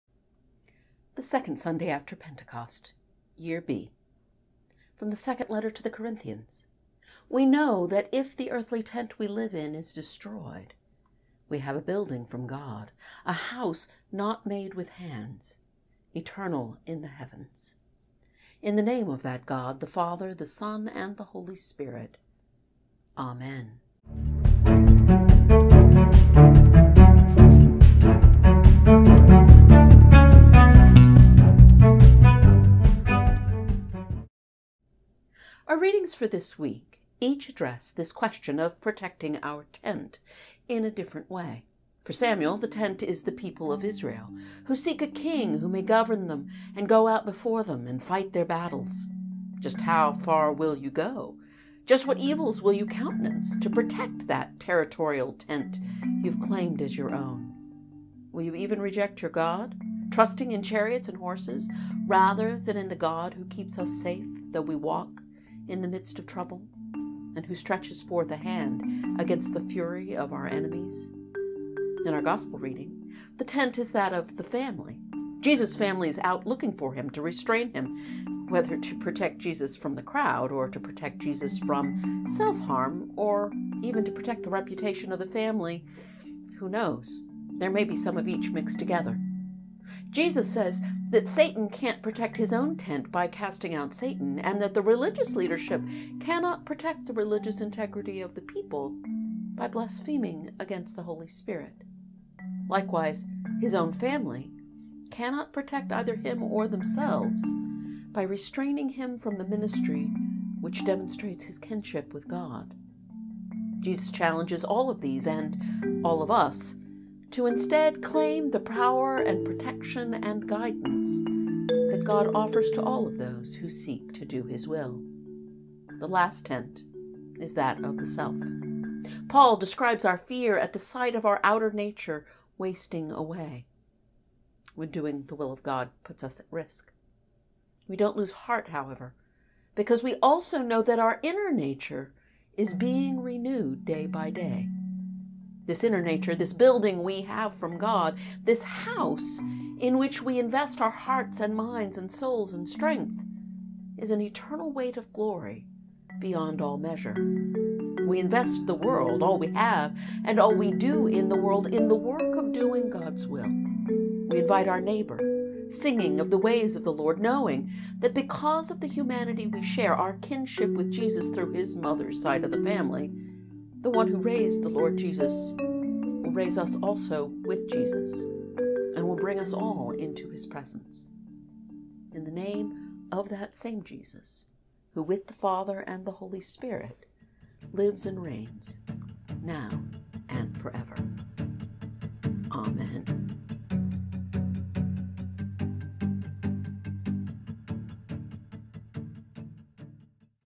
This Earthly Tent
A Sermon on the readings for the 2nd Sunday after Pentecost, Year B:

1 Samuel 8:4-20, 11:14-15
Psalm 138
2 Corinthians 4:13-5:1
Mark 3:20-35

The full version of this sermon was preached as a part of Holy Eucharist at Holy Trinity Episcopal Church in South Bend, IN on June 6, 2021.